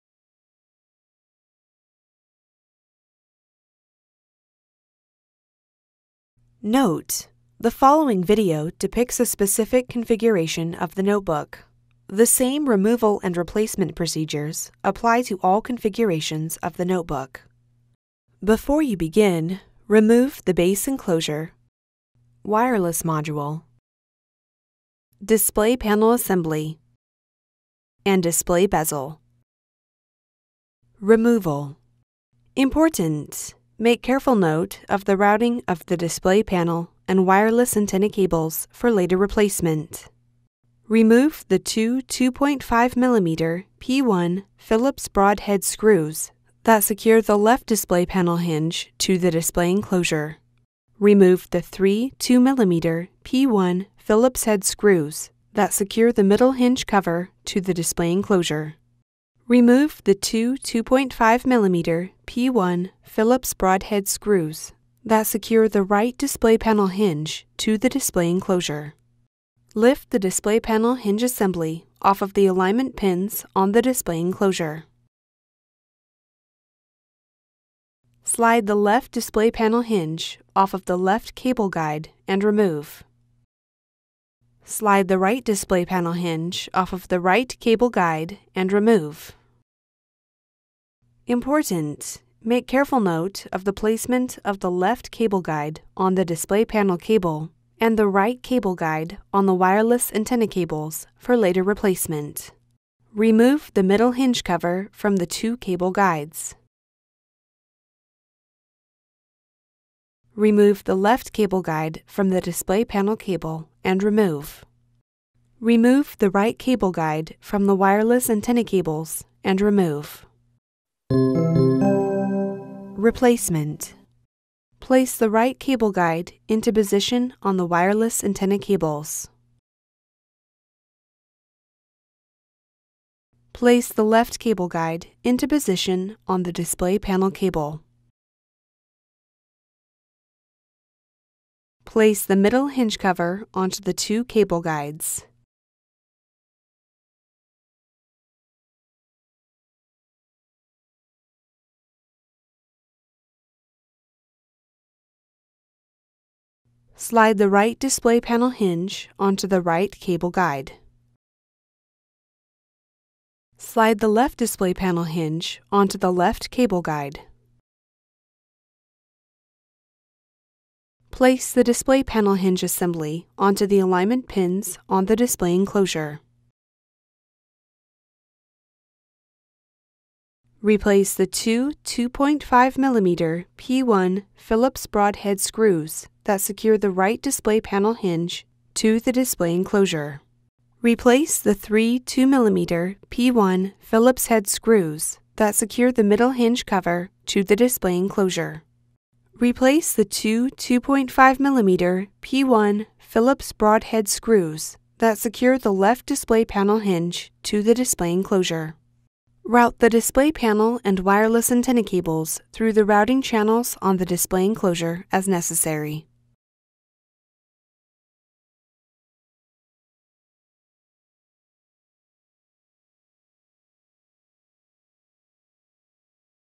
Replace the Display Panel Hinge Assembly | HP Chromebook 13 G1 | HP Support
Learn how to remove and replace the display panel hinge assembly for the HP Chromebook 13 G1.